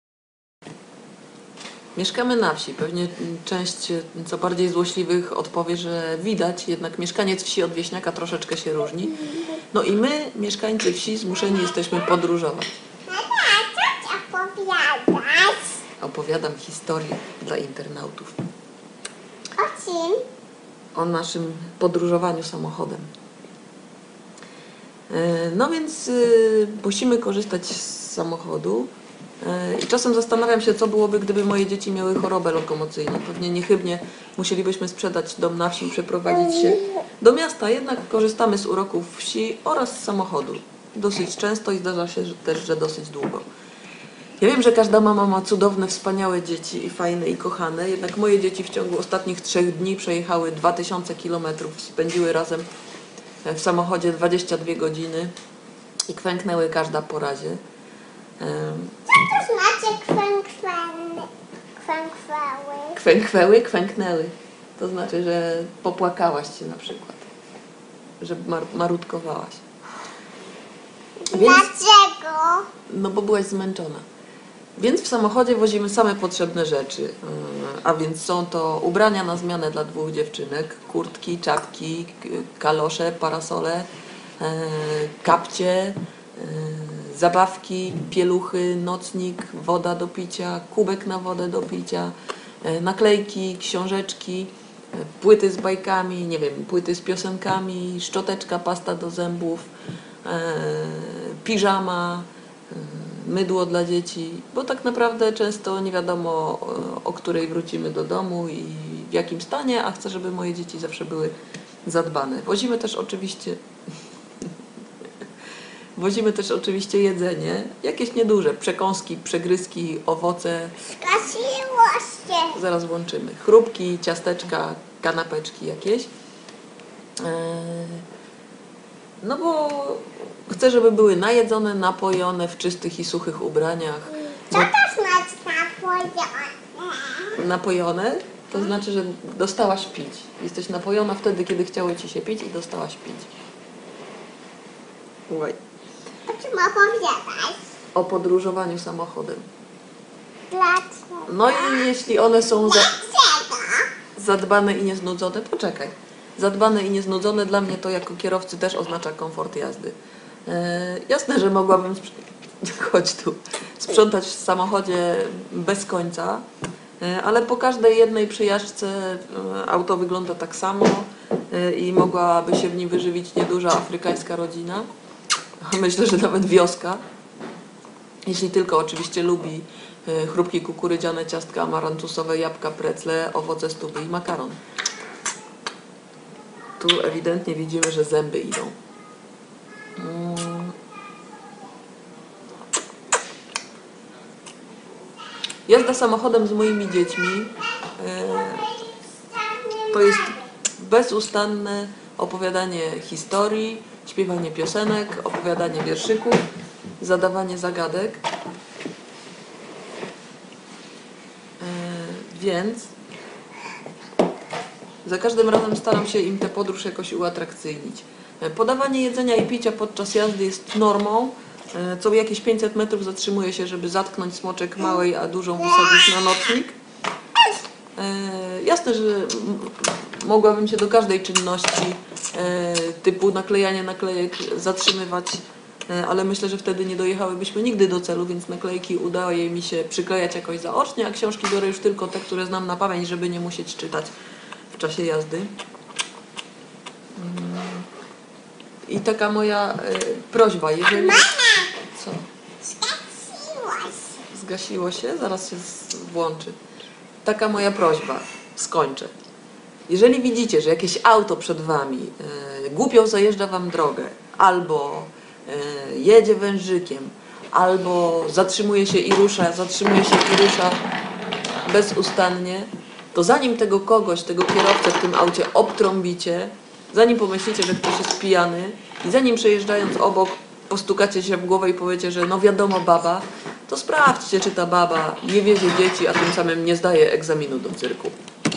Sprawdźcie, czy ta 'baba' nie jedzie z dziećmi! [edziecko]
Magda ma dwie córki i jeździ z nimi często samochodem. Czy trzeba jeszcze coś dodać?